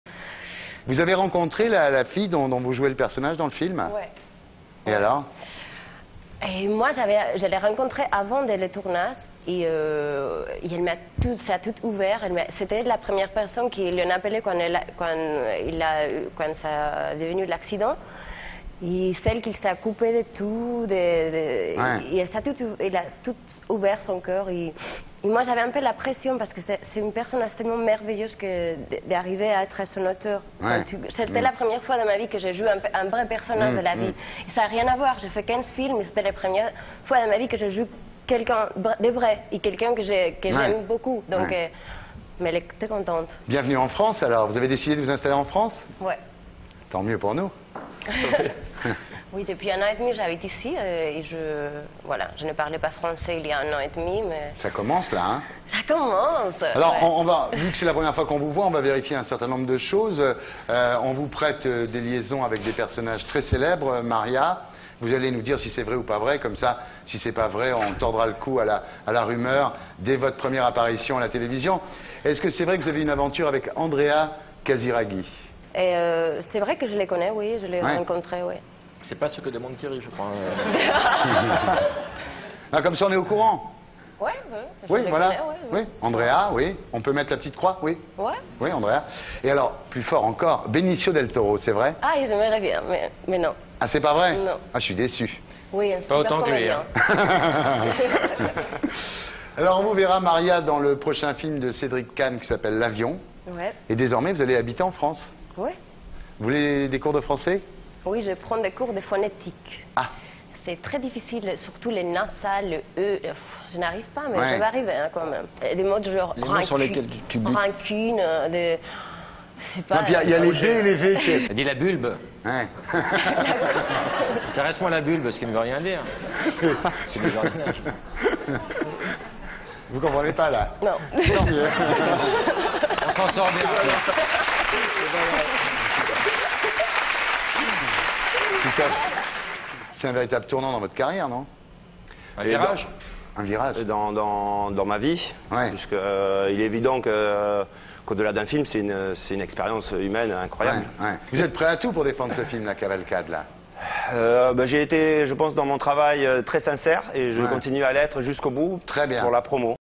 Maria Jurado portrait - Archive INA
Maria JURADO s'entretient sur sa carrière et sur le film "Cavalcade" avec Thierry ARDISSON. Laurent BAFFIE chambre beaucoup cette jeune et jolie comédienne espagnole. Images d'archive INA